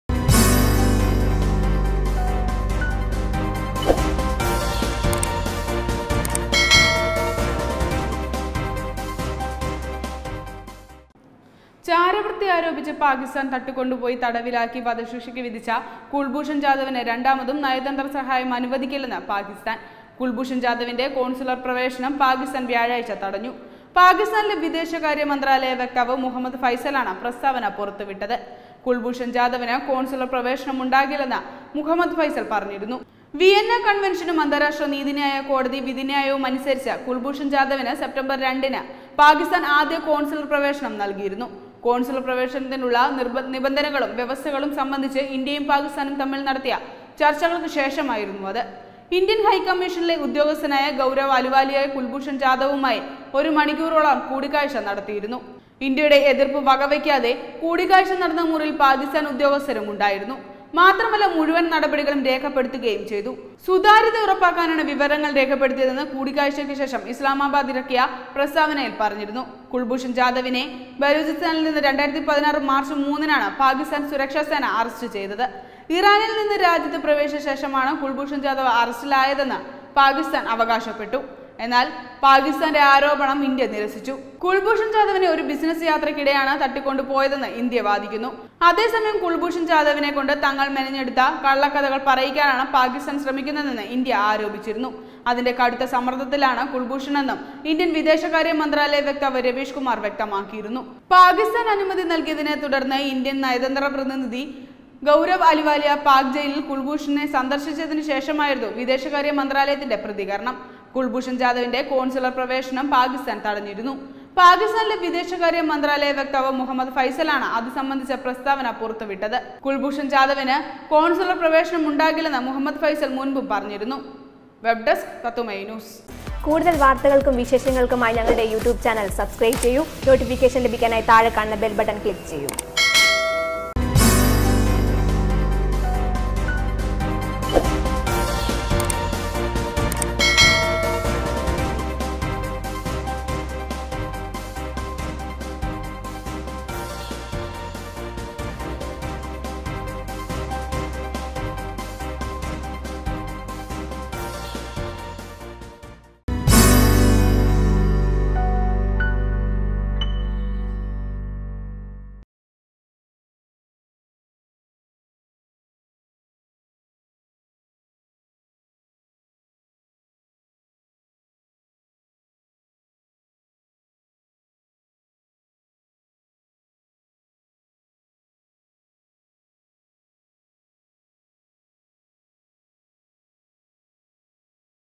'അങ്ങാടിയിൽ തോറ്റതിന് അമ്മയോട് ' - കുൽഭൂഷണെ സമ്മർദ്ദത്തിലാക്കി പാകിസ്താൻ
ചാരവൃത്തി ആരോപിച്ചു പാകിസ്ഥാൻ തട്ടിക്കൊണ്ടുപോയി തടവിലാക്കി വധശിക്ഷക്ക് വിധിച്ച കുൽഭുഷൻ  ജാദവിന് രണ്ടാമതും നയതന്ത്ര സഹായം അനുവദിക്കില്ലെന്ന് പാകിസ്ഥാന്‍. കുല്‍ഭൂഷന്‍ ജാദവിന്റെ കോണ്‍സുലര്‍ പ്രവേശനം പാകിസ്ഥാന്‍ വ്യാഴാഴ്ച തടഞ്ഞു.പാകിസ്ഥാനിലെ വിദേശകാര്യ മന്ത്രാലയ വക്താവ് മുഹമ്മദ് ഫൈസലാണ് പ്രസ്‌താവന പുറത്തുവിട്ടത്.